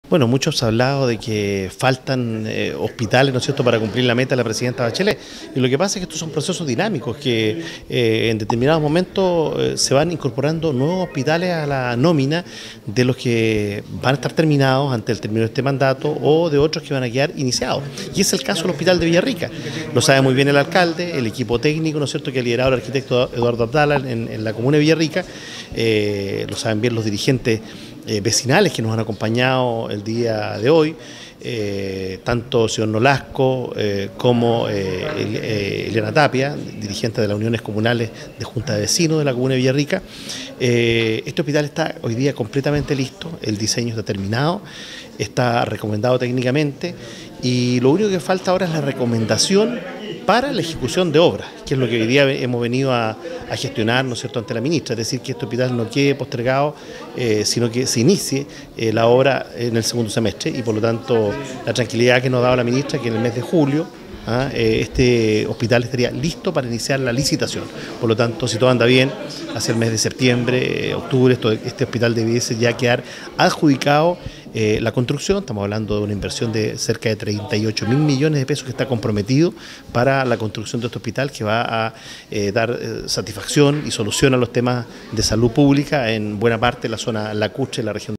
Senador Quintana se reunió con Ministra de Salud para licitación de hospital de Villarica
El parlamentario dijo que la construcción del hospital de Villarica que está con el diseño listo, y lo único que falta es la recomendación para la ejecución de obras para que se inicie su construcción en el segundo semestre de este, por lo que en septiembre debería estar adjudicado.